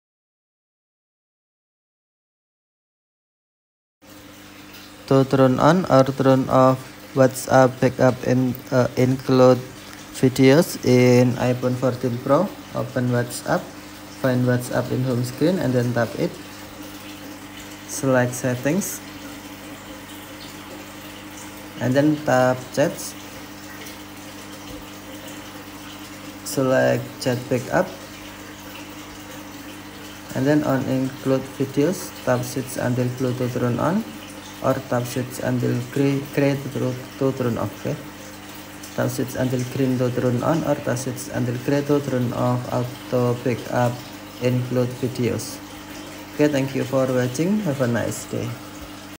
How to turn on or off Whatspp backup include Videos in iPhone 14 Pro
In this video we will learn about How to turn on or off Whatspp backup include Videos. I use  iPhone 14 Pro and WhatsApp version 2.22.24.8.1 in this tutorial